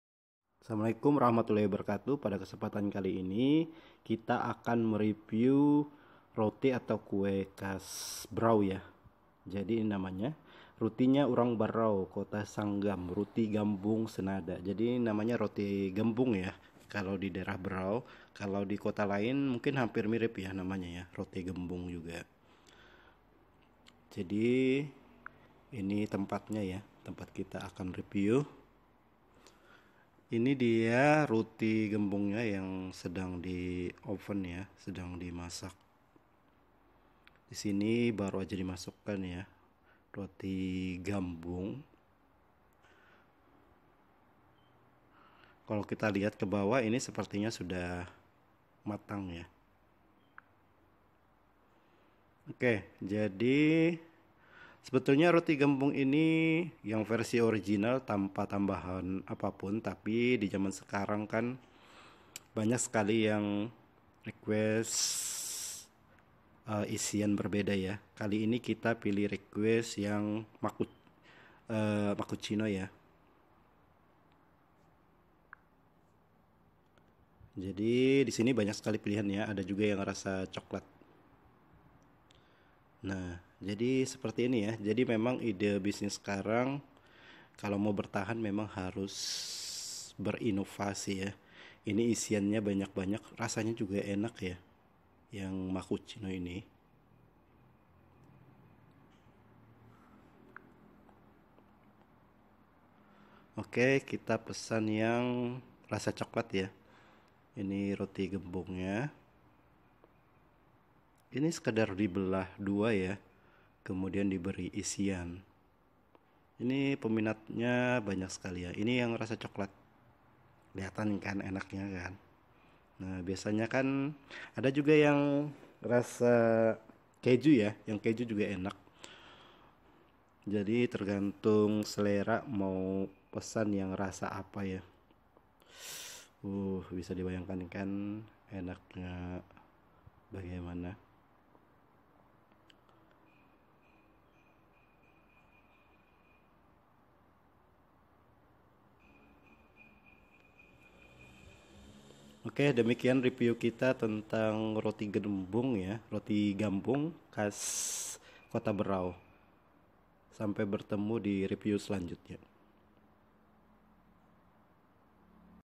Lezatnya Roti Gambung Khas Berau Dengan Rasa Kekinian
Pada kesempatan kali ini kami akan membagikan proses pembuatan roti Gambung. Roti gambung khas berau ini beronovasi dengan menghadirkan rasa Kekinian  yang disukai semua kalangan, baik muda atau tua. Ada varian rasa; mochaccino, coklat, maupun  keju. adapun harganya  adalah : Rp. 15.000/porsi